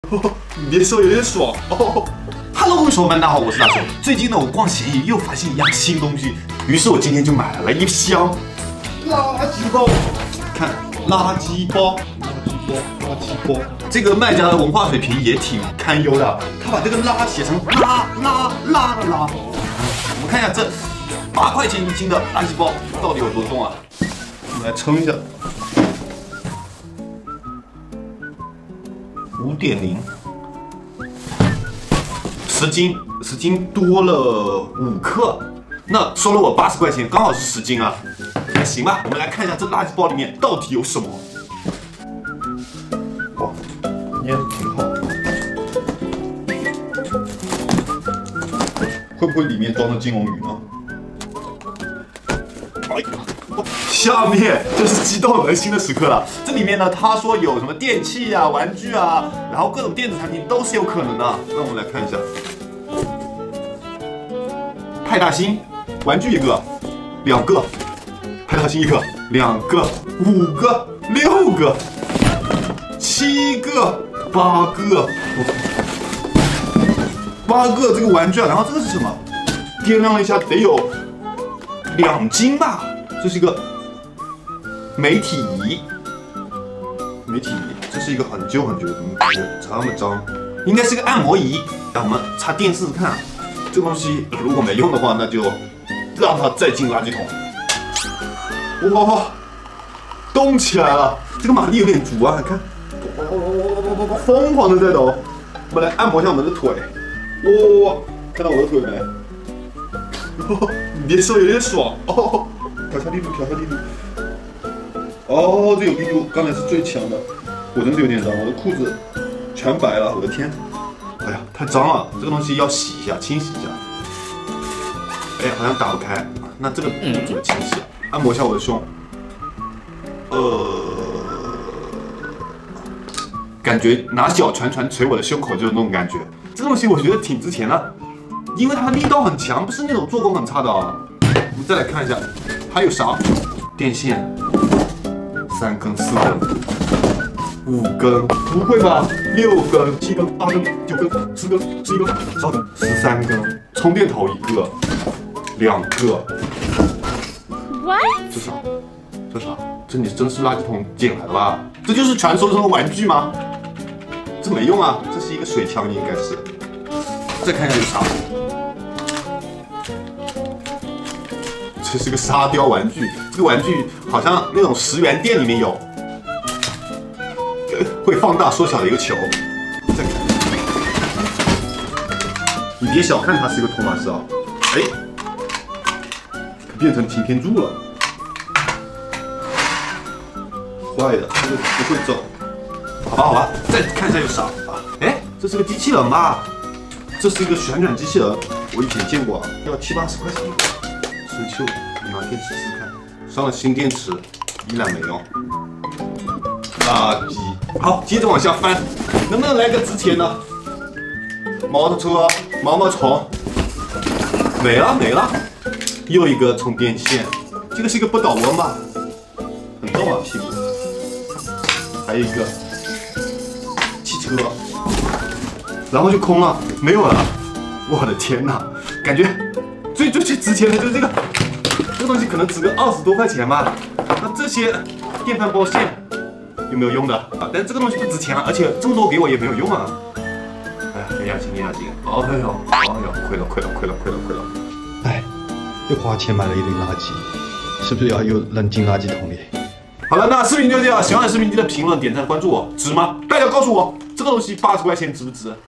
花80元買了10斤垃圾包，壹大堆東西，拆完究竟是血賺還是垃圾呢？
大雄show;花80元買了10斤垃圾包，壹大堆東西，拆完究竟是血賺還是垃圾呢？